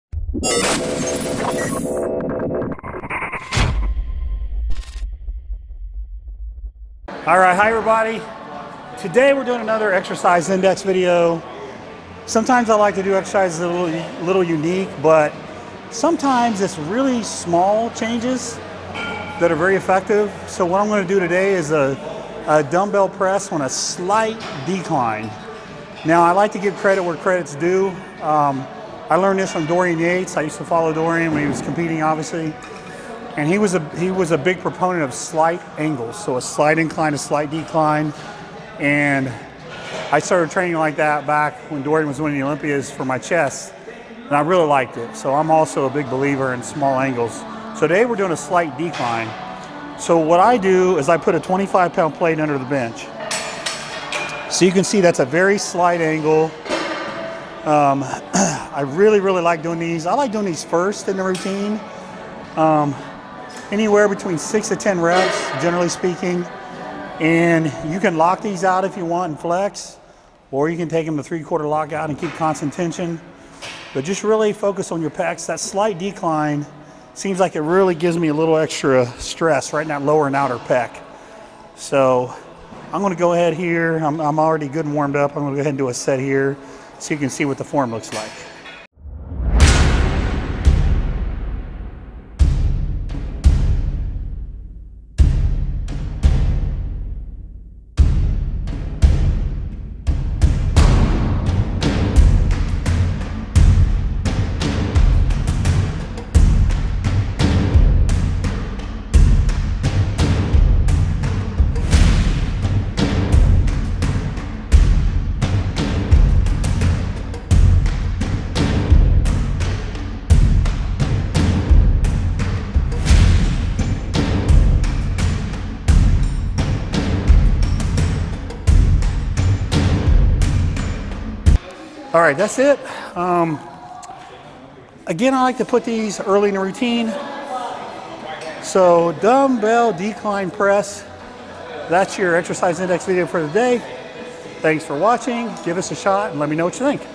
Exercise Index - Slight Decline Dumbbell Press
Today we are doing a slight decline dumbbell press.  I love to do this first thing in my chest day.  You might not think it does much but you have to give it a try.  Let me know how you like it.